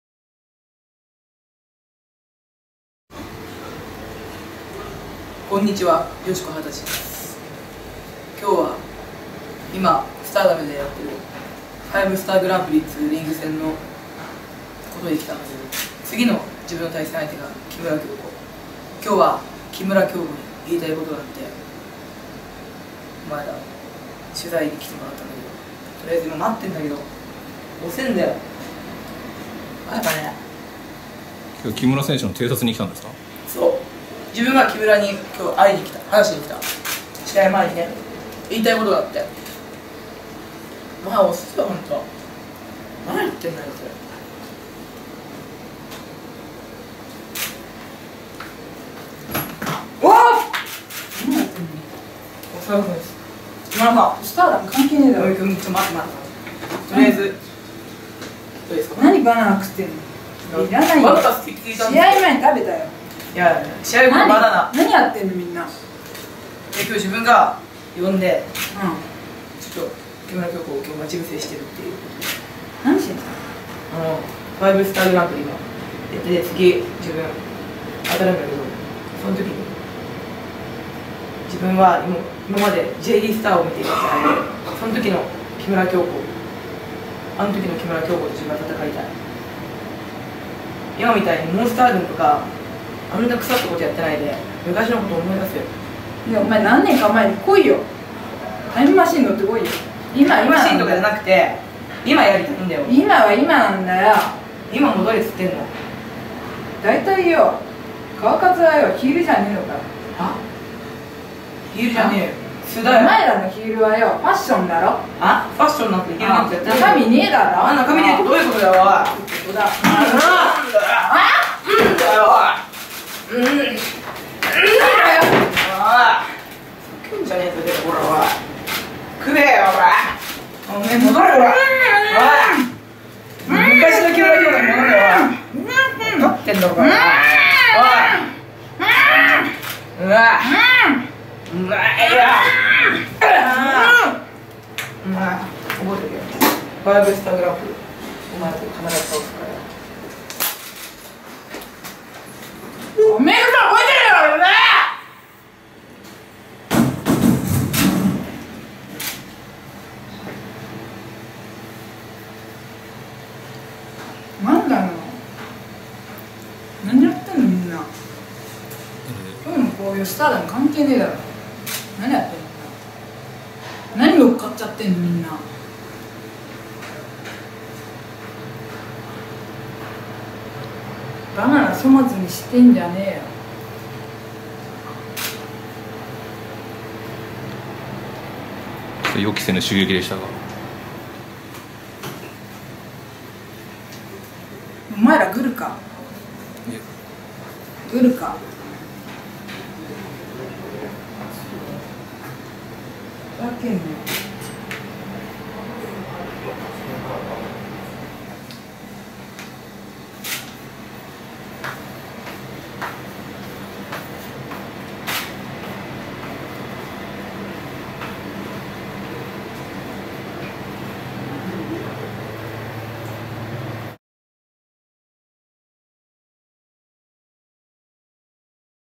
8.28世IV虎が木村響子を襲撃！
８月２８日、世IV虎が「木村響子に話があるから新宿ＦＡＣＥまで来てほしい」と報道陣を呼び出し、夜の新宿ＦＡＣＥへ。この日、同会場では大阪のＶＫＦバトルエンターテインメントの大会が行われており、そこに木村響子も出場していた。
９・８新木場大会の５★ＳＴＡＲ ＧＰ公式戦でシングル初対決を行う木村を偵察に来たという世IV虎は、大会終了後に関係者出入り口で集まった報道陣と共に、持参したバナナを食べながら木村を待つ。ところが、なかなか木村が現れず徐々にイライラしはじめる世IV虎。そこにようやく帰ろうとする木村が現れた.